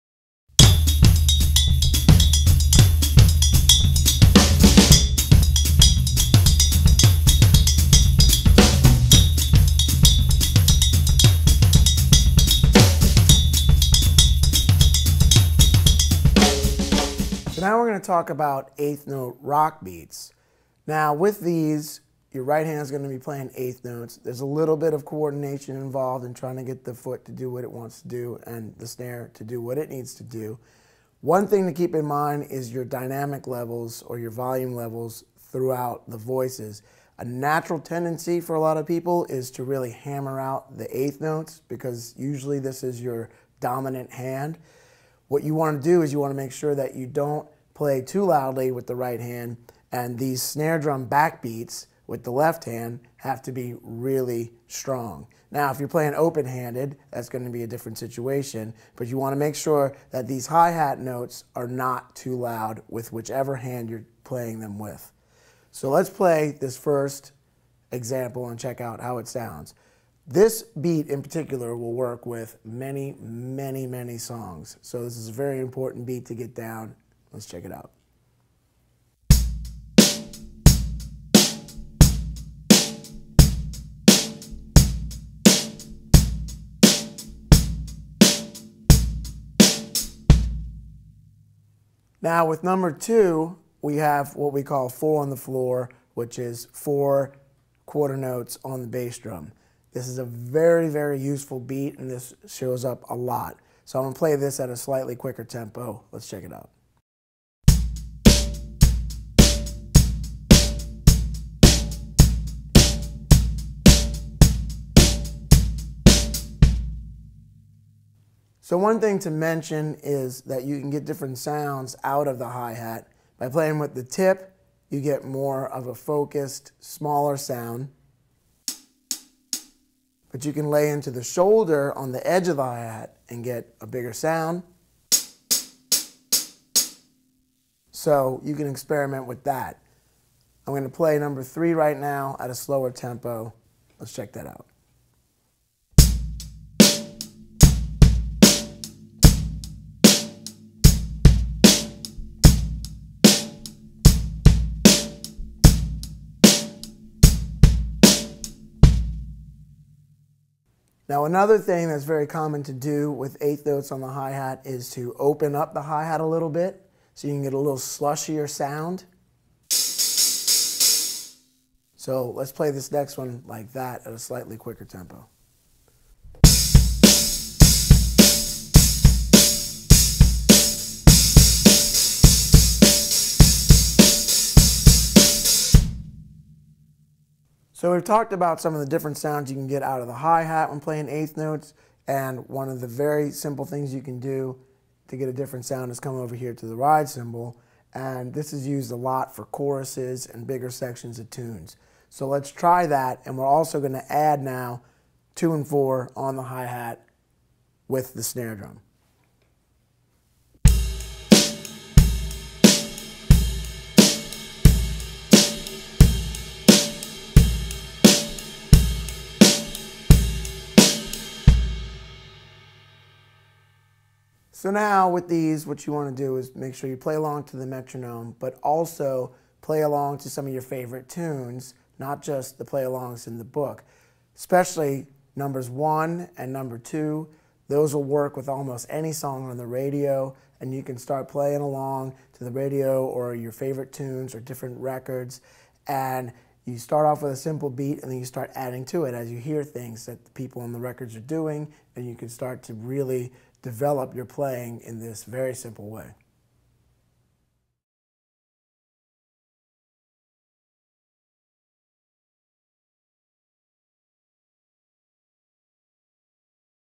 Fresh Approach to Drumset 09: 8th Note Rock Beats
Learn the basics of playing 8th notes on the hi-hat with some bass drum / snare patterns.

Excerpted from "A Fresh Approach to the Drumset" Video Lesson Series